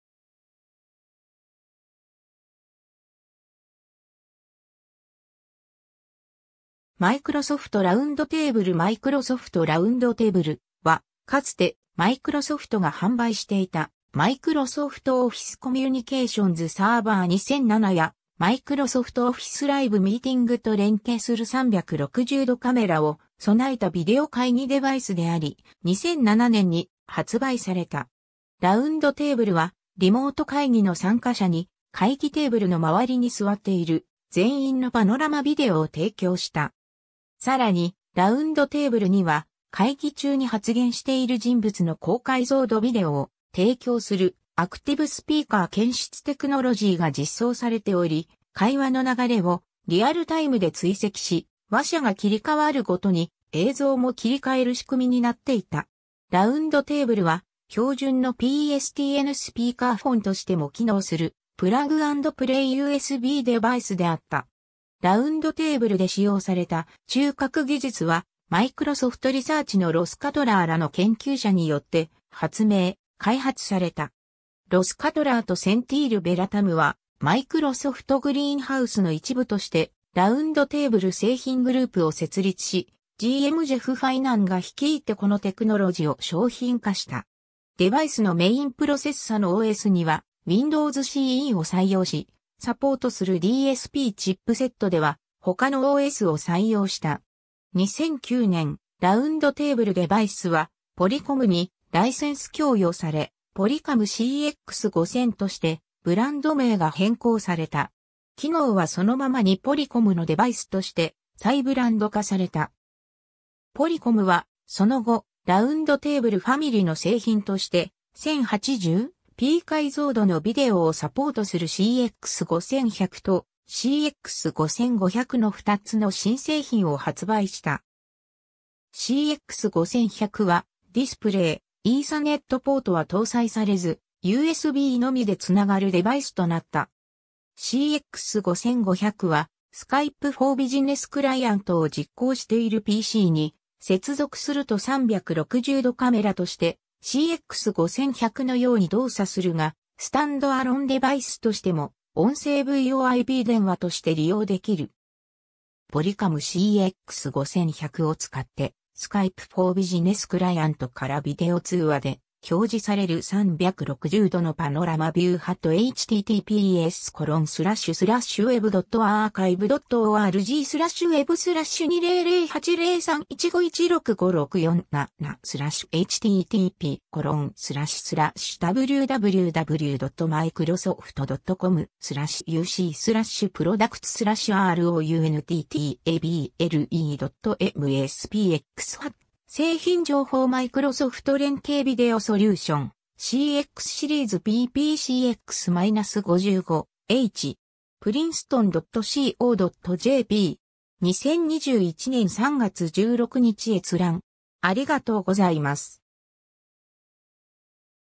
Microsoft RoundTable
テレビ・ビデオ会議
Microsoft RoundTable Microsoft RoundTable (マイクロソフト・ラウンドテーブル)は、かつてマイクロソフトが販売していた、Microsoft Office Communications Server 2007やMicrosoft Office Live Meetingと連携する360度カメラを備えたビデオ会議デバイスであり、2007年に発売された。
 RoundTableは、リモート会議の参加者に、会議テーブルの周りに座っている全員のパノラマビデオを提供した。
さらに、RoundTableには、会議中に発言している人物 (アクティブスピーカー) の高解像度ビデオを提供するアクティブスピーカー検出テクノロジーが実装されており、会話の流れをリアルタイムで追跡し、話者が切り替わるごとに映像も切り替える仕組みになっていた。
 RoundTableは、標準のPSTNスピーカーフォンとしても機能するプラグアンドプレイUSBデバイスであった。
 RoundTableで使用された中核技術は、マイクロソフトリサーチのRoss Cutlerらの研究者によって発明、開発された。
 Ross CutlerとSenthil Velayuthamは、Microsoft Greenhouseの一部としてRoundTable製品グループを設立し、GM JeffFinanが率いてこのテクノロジを商品化した。
デバイスのメインプロセッサのOSにはWindows CEを採用し、サポートするDSPチップセットでは他のOSを採用した。
 2009年、RoundTableデバイスはポリコムにライセンス供与され、Polycom CX5000としてブランド名が変更された。
機能はそのままにポリコムのデバイスとして再ブランド化された。
ポリコムはその後、RoundTableファミリの製品として、1080p解像度のビデオをサポートするCX5100とCX5500の2つの新製品を発売した。
 CX5100はディスプレイ、イーサネットポートは搭載されず、USBのみでつながるデバイスとなった。
 CX5500は、Skype for Businessクライアントを実行しているPCに接続すると360度カメラとしてCX5100のように動作するが、スタンドアロンデバイスとしても音声VoIP電話として利用できる。
Polycom CX5100を使ってSkype for Businessクライアントからビデオ通話で表示される360度のパノラマビュー ^ ^ “製品情報 マイクロソフト連携ビデオソリューション CX シリーズ - PPCX-55H”. princeton.co.jp. 2021年3月16日閲覧。